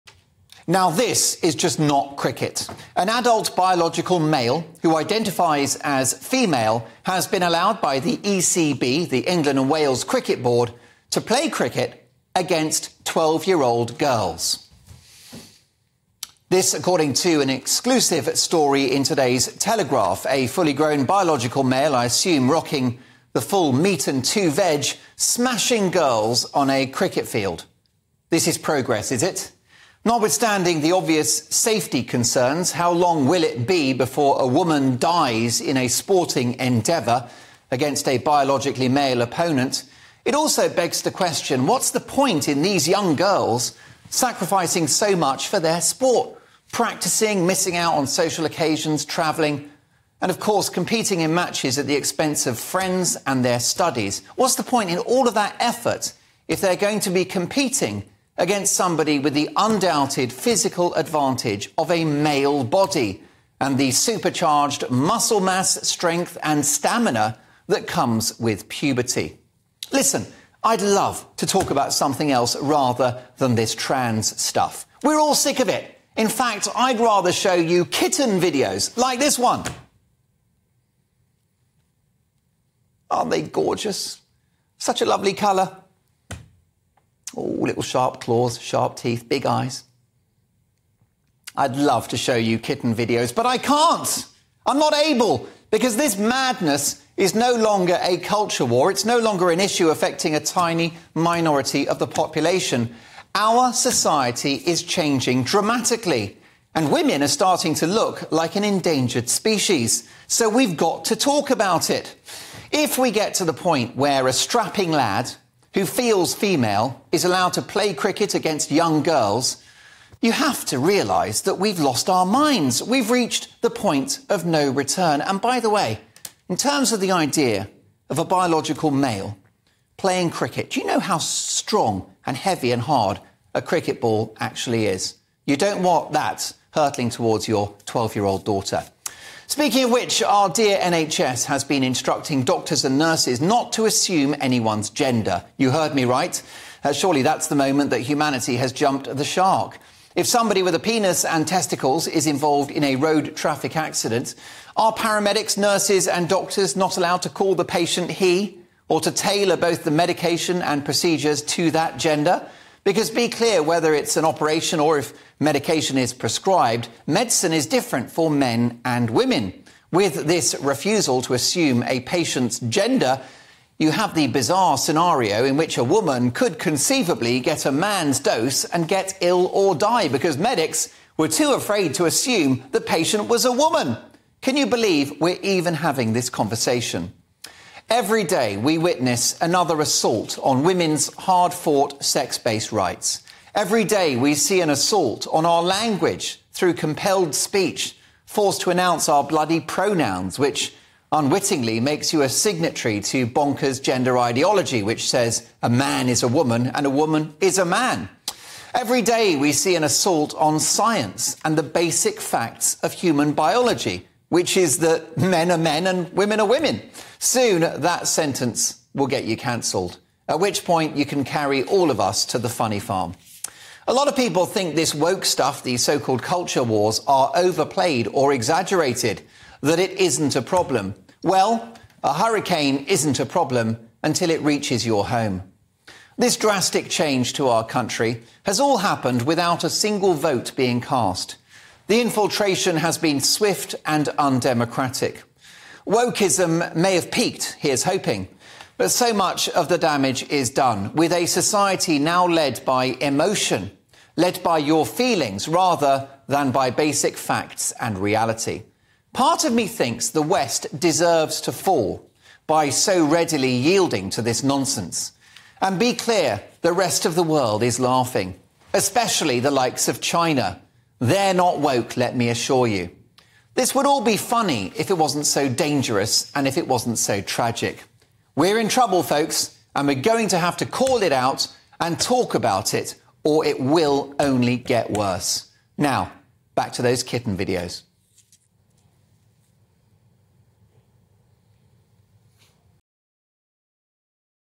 Trans woman allowed to play cricket against young girls | 'women look like an endangered species!'
'How long will it be before a woman dies in a sporting endeavour against a biologically male opponent... women are starting to look like an endangered species.'

Mark Dolan shares his thoughts after a biological man who identifies as a female has been allowed to competitively play cricket again 12-year-old girls.